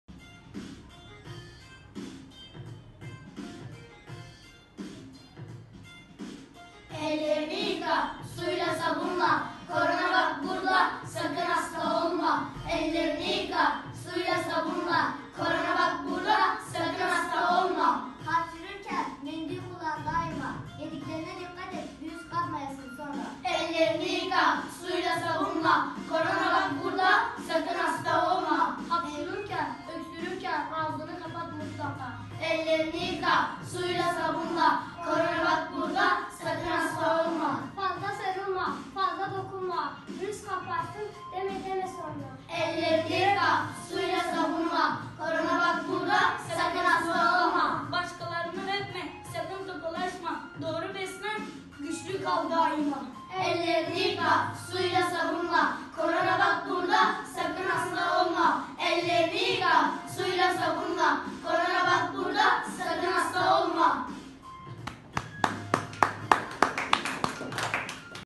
Koronavirüs İle İlgili Şarkı Çıkardı
Siirt Alparslan İkokulu öğrencileri, koronavirüse dikkat çekmek için şarkı yaptı.